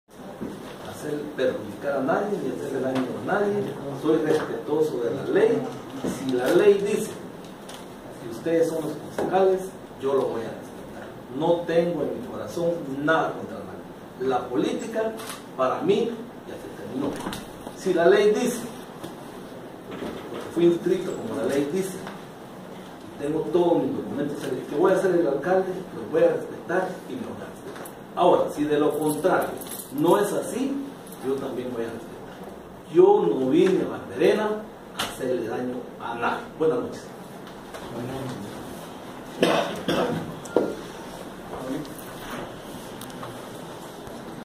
Rubelio Recinos se aferra a alcaldía en Barberena
La transmisión de mando municipal en Barberena, Santa Rosa, no se concretó el pasado 15 de enero, como lo ordena la ley. El alcalde saliente, Rubelio Recinos, se mantiene en el poder gracias a un amparo exprés otorgado por el juez Noé Rivas Chamo, resuelto y notificado ese mismo viernes.  Continuar leyendo